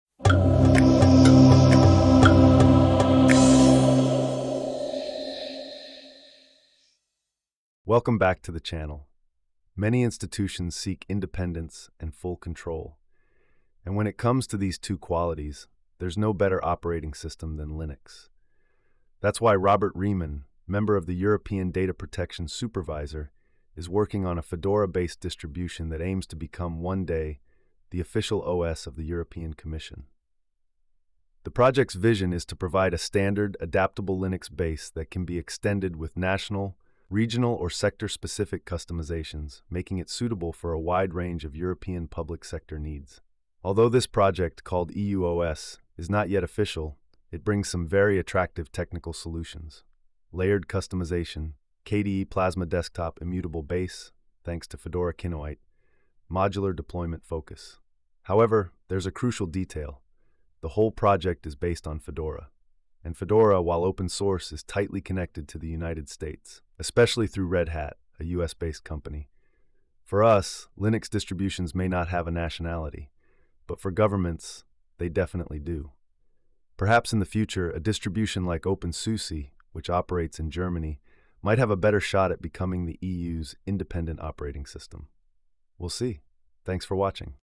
An Operating System for Europe… Without Europe
The European Union is exploring its own Linux-based operating system… but it's built on Fedora, a U.S.-linked project. Can we really talk about digital sovereignty this way? Let’s break down the contradictions of EU OS and what it means for open source in Europe.